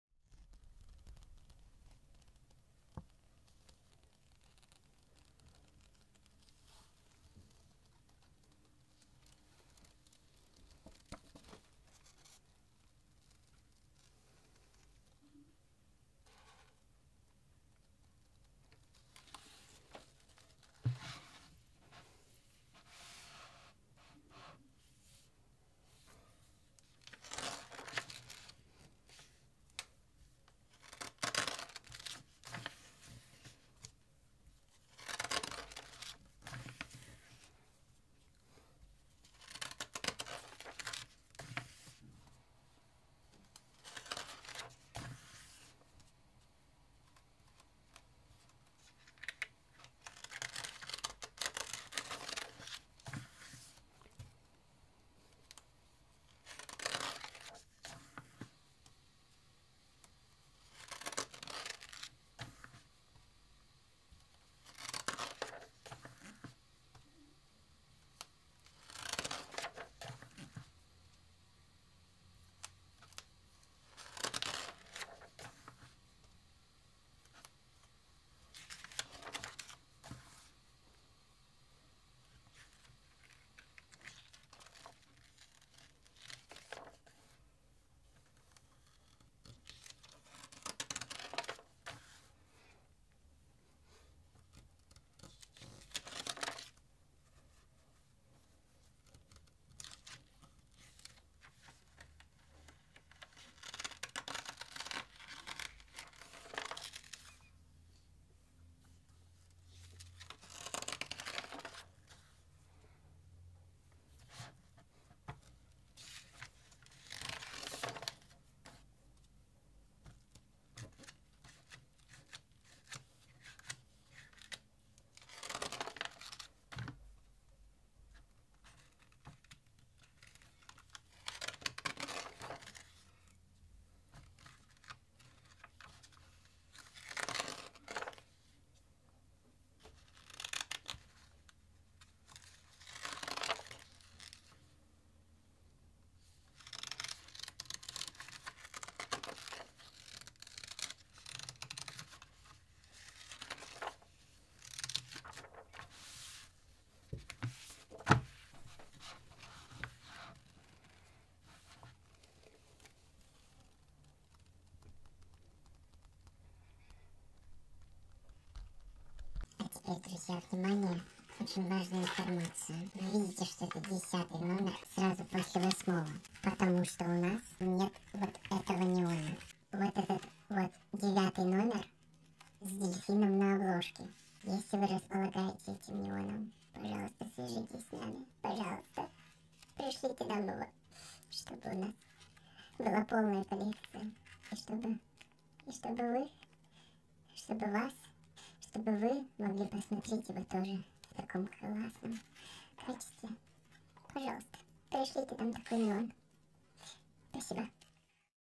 Журнал НЕОН (№10 Июнь 2001): Тату, Дискотека Авария, Премьер-Министр, Destiny's Child и другие
Перед вами журнал НЕОН №10 2001, на обложке которого — t.A.T.u.!

Внутри: Depeche Mode, Lou Bega, Стрелки, Demo, Евровидение, Конец фильма, ТТ, Total, Alcazar, Чичерина, Братья Улыбайте и другие!

Пожалуйста, не скупитесь на ЛАЙКИ и КОММЕНТАРИИ, ведь именно они мотивируют нас становится лучше, чаще и полезнее!

Наш инстаграм — alsblestvia